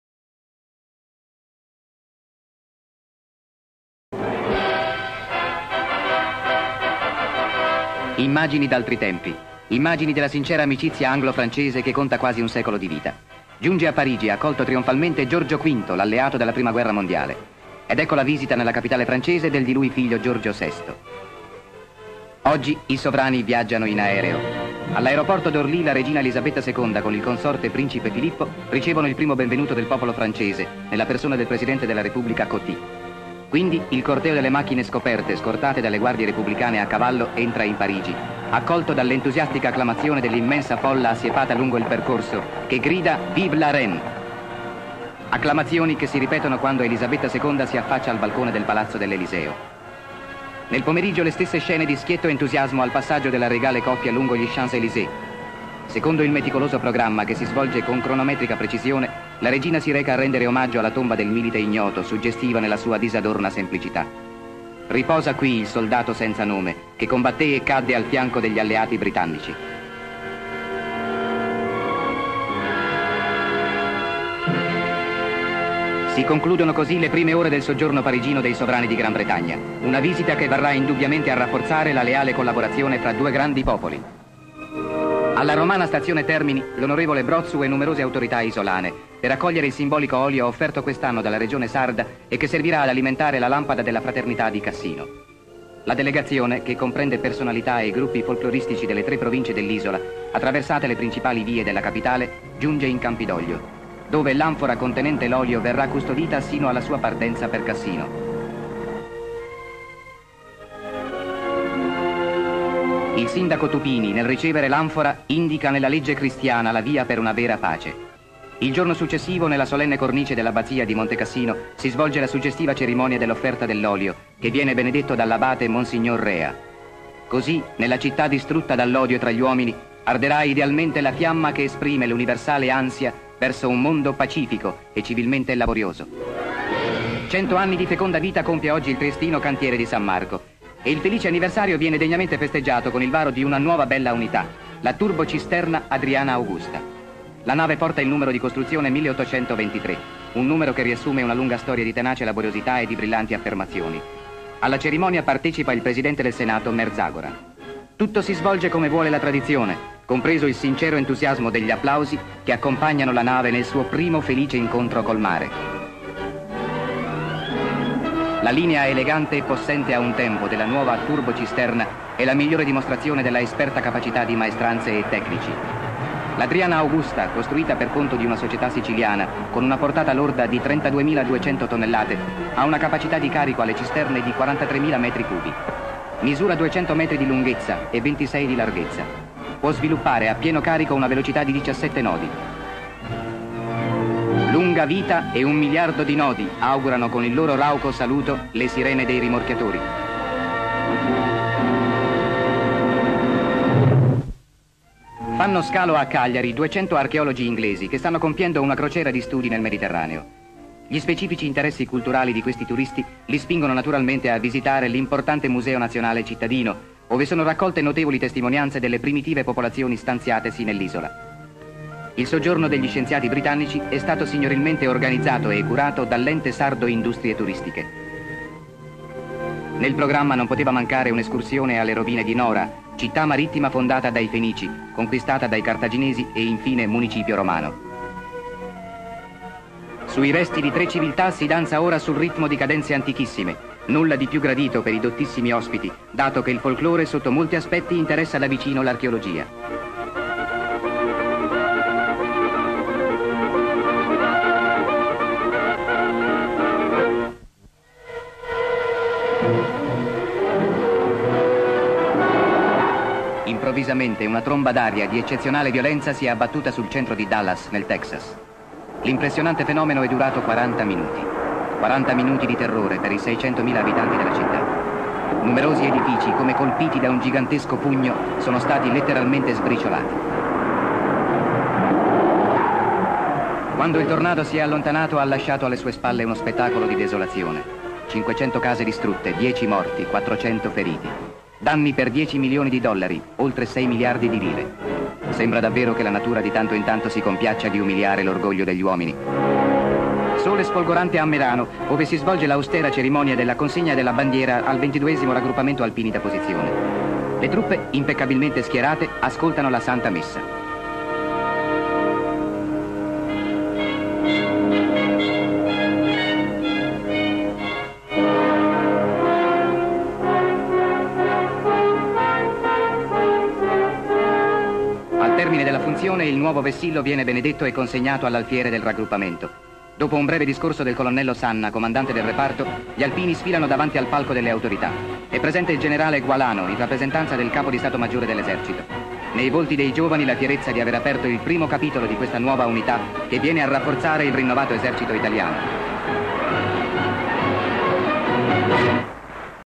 Notizie di cronaca
"Parigi La Repubblica francese tributa accoglienze trionfali alla Regina d'Inghilterra"  "Notizie di cronaca  Roma-Cassino: Offerto dalla Regione Sarda l'olio che alimenterà la simbolica lampada della fraternità  Trieste: i cantieri S. Marco festeggiano i loro cento anni di attività con il varo di una grande petroliera  Sardegna: 200 archeologi inglesi visitano le antichità dell'isola  Texas: un pauroso 'tornado' semidistrugge la città di Dallas  Merano: consegnata la bandiera al 22° Raggruppamento Alpini da posizione"
   Descrizione sequenze:Parigi ; Immagini di Repertorio: ; corteo reale scortata dalla cavalleria attraversa ponte di Parigi ; Vittoria Maria sale su carrozza ; Giorgio V sale su una carrozza ; Giorgio VI scende da carrozza del treno, riceve gli omaggi di personalità, la regina madre segue Giorgio VI ; la cavalleria corre lungo ampio viale alberato ; la regina Elisabetta, Filippo scendono dall'aereo all'aeroporto ; bandiere sventolano su pennoni ; la banda suona all'aeroporto, picchetto d'onore ; il corteo di autorità attraversa piazzale all'aeroporto ; il corteo reale scortato da guardia reale giunge a Parigi ; la folla assiepata lungo il percorso del corteo reale ; La regina, il marito e Coty affacciati al balcone dell'Eliseo ; veduta dall'alto degli champs Elysées ; la folla assiste al passaggio della famiglia reale lungo i campi elisi ; l'arco di trionfo ; bandiere sventolano al vento ; la regina e il marito depongono corona al monumento al milite ignoto ; la famiglia reale davanti al monumento ; una fiaccola arde, le corone di fiori ; il corteo reale preceduto da macchina con a bordo operatori ripercorre i Campi elisi, la regina saluta dalla macchina. ; Roma ; stazione Termini: militari e civili con gonfaloni lungo la banchina ; Brotzu parla con autorità religiose e civili ; il treno in partenza ; piccolo corteo aperto da gonfalone, portato da uomini e donne in abiti folcloristici ; corteo di camion carichi di manifestanti e gonfaloni attraversa via dei fori imperiali, piazza Venezia ; il corteo si ferma al Campidoglio ; un carabiniere sale le scale del campidoglio portando un'anfora ; un sindaco consegna a Tupini l'anfora ; donne in abiti tradizionali assistono alla cerimonia ; corteo con gonfaloni in una terrazza dell'Abbazia ; autorità militari e civili in processione in una sala ; l'anfora ed oggetti su un tavolo ; cerimonia religiosa, un carabiniere sorregge l'anfora ; monsignore Rea versa l'olio dell'anfora ; due donne sorreggono un cuscino, monsignore accende una candela posta su cuscino ; il gonfalone di Cassino. ; Trieste: ; cantieri navali ; la turbocisterna Augusta sui sostegni ; le maestranze rifiniscono lo scafo ; veduta dall'alto di strada, bandiere sventolano ; corteo di personalità giunge ai cantieri, Merzagora nel corteo aperto da signora con mazzo di fiori ; veduta dall'alto della cerimonia, la strada gremita di gente, il palco delle autorità ; donna con il mazzo di fiori sul palco guarda la nave ; veduta dalla nave che entra in mare della folla riunita lungo la banchina ; la nave scivola in mare, l'equipaggio saluta ; lo scafo nell'acqua ; la banchina si allontana, veduta dei cantieri ; la popolazione si affaccia dalle transenne ; i rimorchiatori scortano la nave, gente sulla banchina ; Cagliari: ; un traghetto nel porto ; viaggiatori scendono dal traghetto ; la facciata del museo di Cagliari ; i visitatori tra i reperti in mostra nelle sale ; i visitatori tra i resti archeologici di Nora ; danze folcloristiche nelle rovine di Nora. ; Dallas: ; tromba d'aria ; una macchina si dirige verso la tromba d'aria ; il vortice ; oggetti spazzati via dal tornado ; poliziotti ed abitanti si aggirano tra macerie ; case abbattute. ; Merano: ; militari passano in rivista gli alpini schierati in un viale ; gli alpini sull'attenti davanti ad altare all'aperto ; un drappello avanza portando bandiera ; Sanna srotola la bandiera offerta da alpino ; religioso benedice il vessillo preso in consegna da alpino ; Sanna pronuncia un discorso ; gli alpini sfilano davanti al palco delle autorità ; la popolazione assiste alla sfilata e applaude. ;